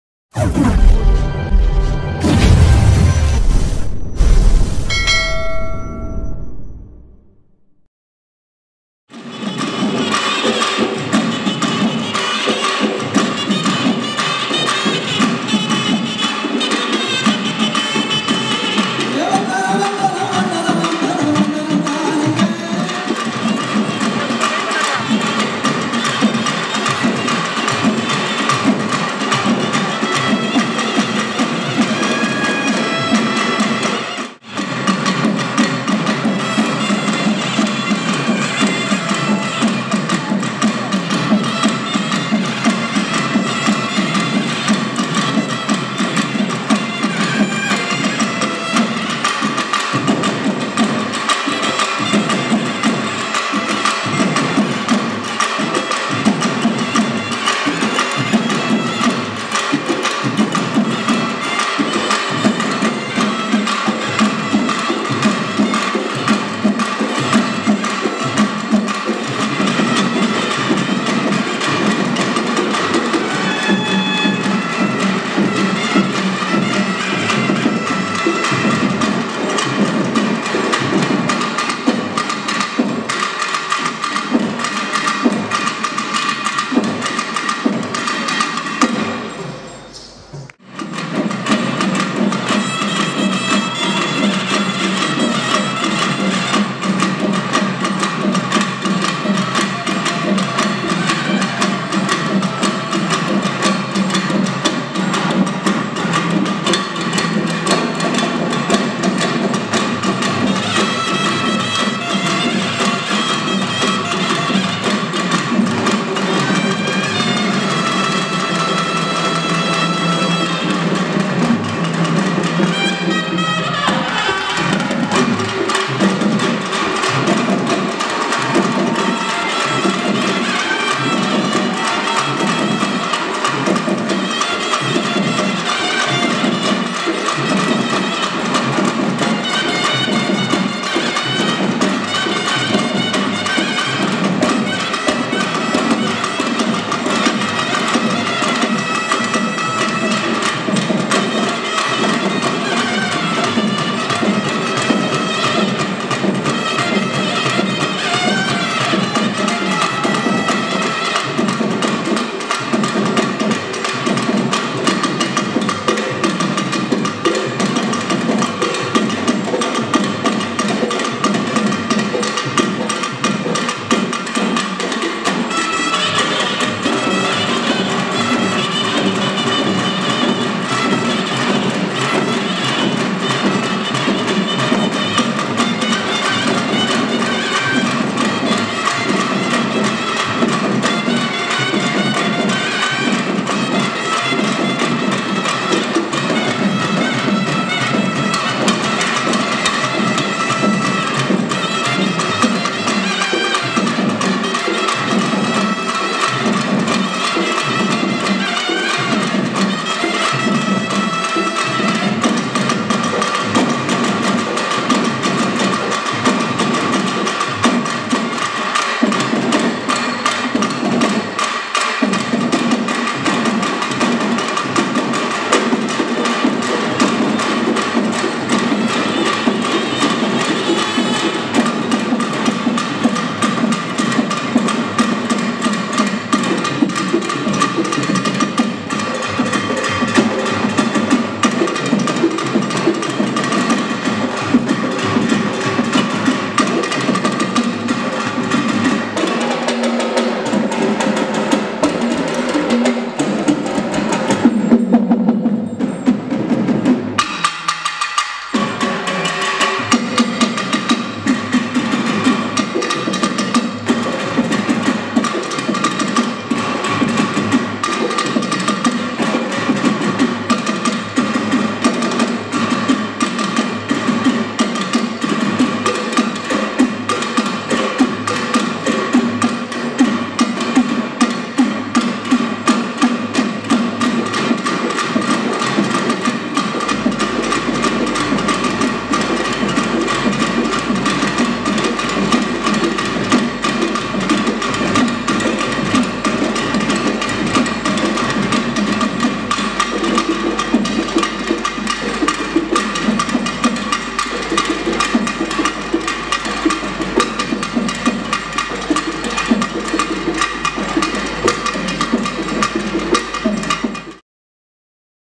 Kashi Tamil Sangamam 2022 | डमी हार्स, बुल डांस व मयूर नृत्य BHU, Varanasi
Amazing dance performance by Tamil artists. First time dekha aisa performance🤗🤗.. Sham me Friends agar aap free ho to ja sakte hai program ko dekhne ke lye koi entry fee nahi hai. 16 December tak ye program chalne wala hai Banaras Hindu University, Varanasi me.